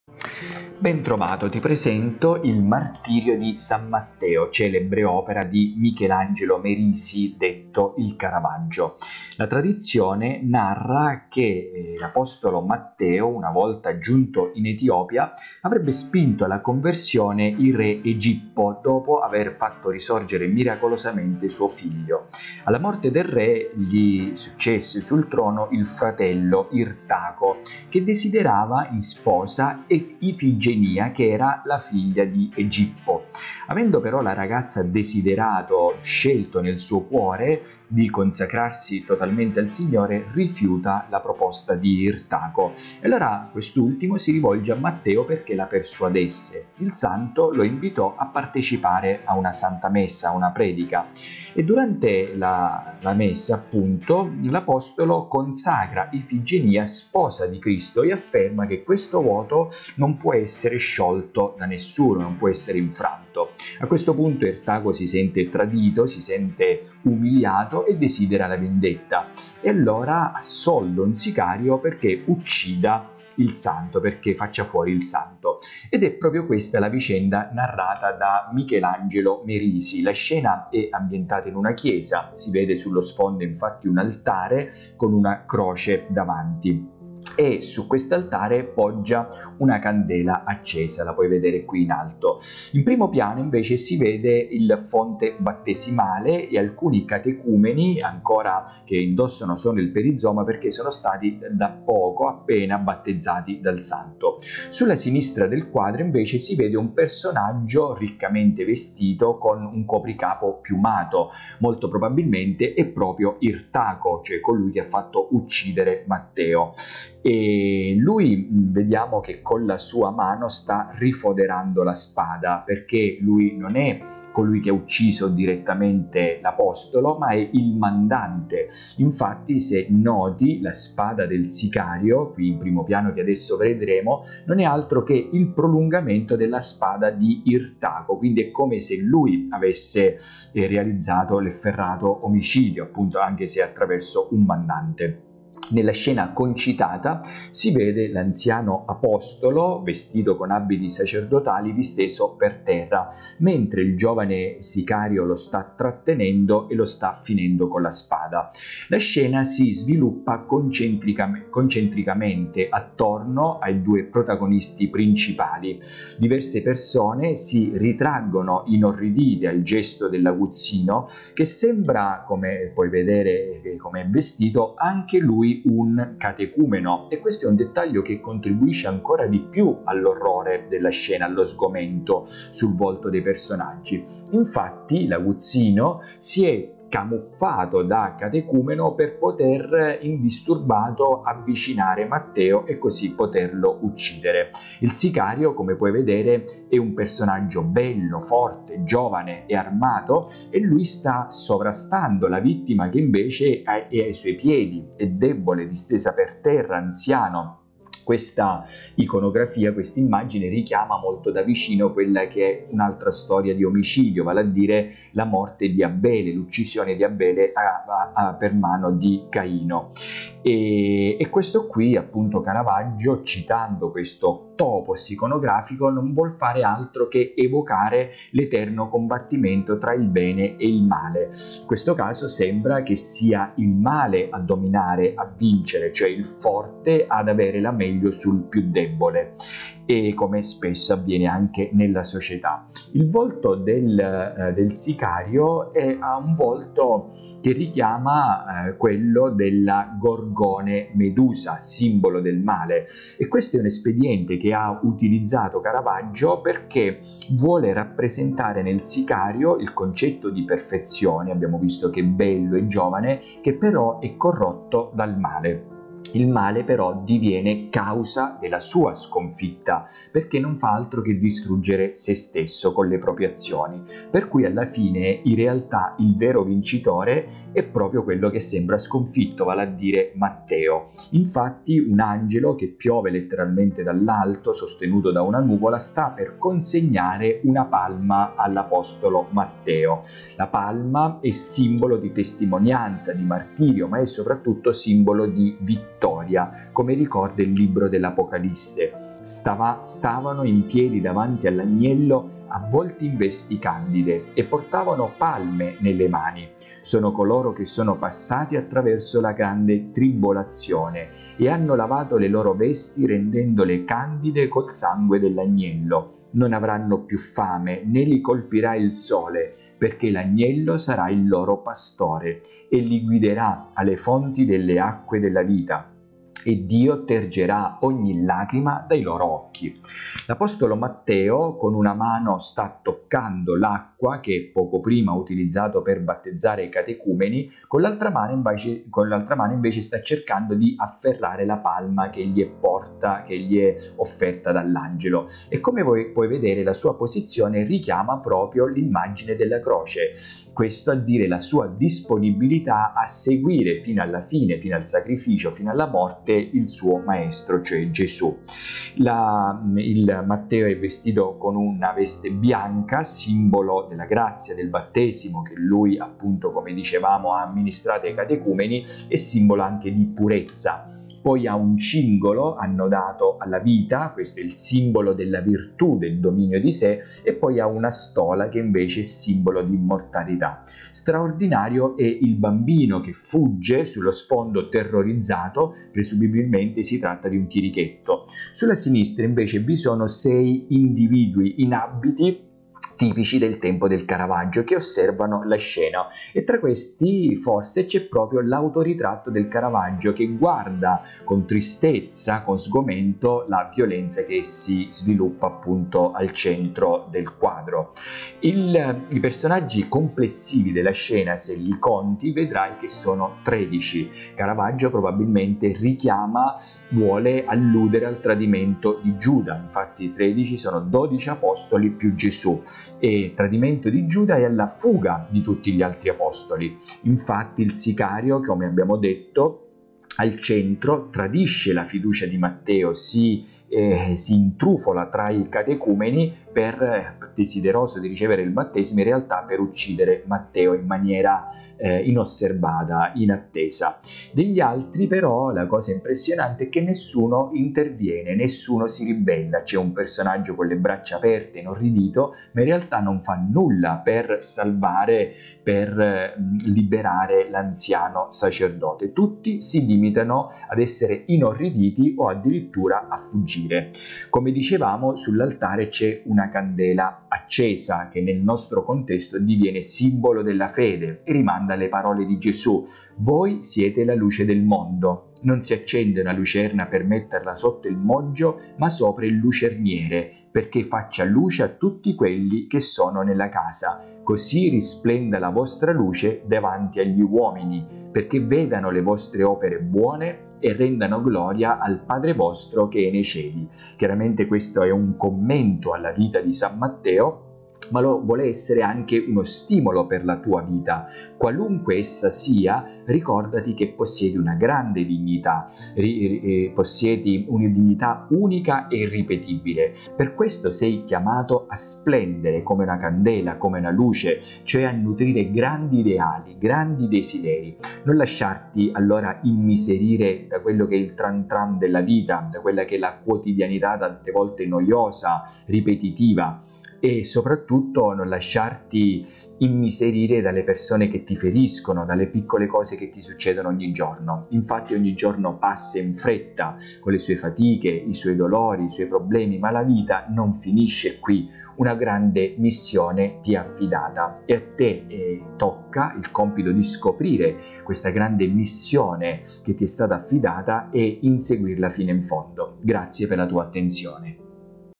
Caravaggio - Martirio di San Matteo. Commento iconografico spirituale a cura di Alessio Fucile
La vicenda è ambientata in una chiesa, per cui sullo sfondo appare un altare inciso con una croce e su cui poggia una candela accesa; in primo piano si intravedono la vasca battesimale e alcuni catecumeni che indossano solo il perizoma. Sulla sinistra del quadro si può distinguere un personaggio dallo sguardo impietoso e con un ricco cappello piumato: forse si tratta del re Irtaco, mandante dell'omicidio, in quanto la sua mano rinfodera la spada a voler significare che da lui è giunto l'ordine dell'assassinio e che il re in prima persona ha lasciato compiere il delitto a un suo sicario. Ciononostante, la sua spada e quella dell’assassino si intersecano tanto che la seconda pare il prolungamento della prima, ad intendere che è comunque stata la spada del re a uccidere il santo.
Nella scena concitata si vede l’anziano apostolo vestito con abiti sacerdotali e disteso per terra mentre il giovane sicario lo blocca e lo finisce con la spada. La scena si sviluppa concentricamente proprio intorno a queste due figure centrali: diverse persone infatti si ritraggono inorridite di fronte al gesto dell’aguzzino che sembra essere anche lui uno dei catecumeni, dettaglio che contribuisce ad aumentare lo sgomento disegnato sui volti dei personaggi presenti. In realtà, l’assassino decide volutamente di camuffarsi da catecumeno per poter avvicinare il santo e ucciderlo; il sicario è giovane, bello, forte, armato e sovrasta la sua vittima debole e anziana, distesa ai suoi piedi. Proprio a simboleggiare la sua preponderante superiorità indossa la tenia, la benda intorno alla fronte che indossavano anche gli atleti vincitori nell'antica Grecia. La posa che contrappone il sicario a Matteo è la medesima di Caino che uccide Abele, evocando così l'eterno combattimento fra il bene e il male. In questo caso sembrerebbe che sia il male a vincere nel dominio del forte sul debole, come spesso avviene nella società : il giovane sicario ha un volto che il Merisi ha reso simile a quello della gorgone Medusa, simbolo del male, al fine di rappresentare nel sicario il concetto di perfezione corrotta dal male. Ma il male diviene causa della propria sconfitta distruggendo sé stesso con le sue proprie azioni, per cui alla fine allora il vero vincitore non è colui che sembra essere tale, ma il santo che sta per ricevere la palma dall’angelo che piove dall’alto sorretto da una nuvola. La palma è simbolo di testimonianza e soprattutto di vittoria, come ricorda anche l’Apocalisse: «Stavano in piedi davanti all'Agnello, avvolti in vesti candide, e portavano palme nelle mani. Sono coloro che sono passati attraverso la grande tribolazione e hanno lavato le loro vesti rendendole candide col sangue dell'Agnello. Non avranno più fame, né li colpirà il sole, perché l'Agnello sarà il loro pastore e li guiderà alle fonti delle acque della vita. E Dio tergerà ogni lacrima dai loro occhi». L’apostolo Matteo con una mano tocca l'acqua del fonte battesimale e con l'altra vuole afferrare la palma; con il suo corpo forma una croce, a simboleggiare il suo desiderio di seguire Gesù sino alla morte; ha la veste sacerdotale bianca, simbolo della grazia del Battesimo che sta amministrando ai catecumeni e della purezza di cuore; il cingolo annodato alla vita è simbolo della virtù del dominio di sé, la stola invece è simbolo dell'immortalità. Straordinario è il bambino che fugge terrorizzato, presumibilmente si tratta di un chierichetto. Sulla sinistra sei individui in abiti del tempo del Caravaggio osservano la scena: tra questi c’è forse lo stesso artista che si rappresenta sullo sfondo mentre osserva sgomento e rattristato la scena di violenza.
I personaggi complessivi nella scena sono tredici, numero che rimanda ai dodici apostoli più Gesù: il Merisi sembra così voler alludere al tradimento di Giuda e alla fuga degli altri apostoli. Il sicario infatti, come abbiamo detto, tradisce la fiducia di Matteo perché si infiltra tra i catecumeni, fingendosi desideroso di ricevere il battesimo per poi colpirlo in modo inatteso; degli altri, nessuno interviene né si ribella al male, uno allarga le mani per lo stupore ma non tenta di salvare l'anziano e indifeso Matteo. Tutti si limitano a guardare o addirittura a fuggire.
Come dicevamo, sull’altare c’è una candela accesa che nel contesto diviene simbolo della fede e rimanda alle parole di Gesù: «Voi siete la luce del mondo; non si accende una lucerna per metterla sotto il moggio, ma sopra il lucerniere perché faccia luce a tutti quelli che sono nella casa. Così risplenda la vostra luce davanti agli uomini, perché vedano le vostre opere buone e rendano gloria al vostro Padre che è nei cieli». Qualunque sia la tua vita, ricordati che possiedi una enorme dignità che ti rende unico e irripetibile: per questo sei chiamato a splendere, a nutrire grandi ideali e grandi desideri. Ogni giorno passa in fretta con le sue fatiche, le sue gioie e dolori ma la tua vita non finisce qui: una grande missione ti è affidata. Scoprila e inseguila!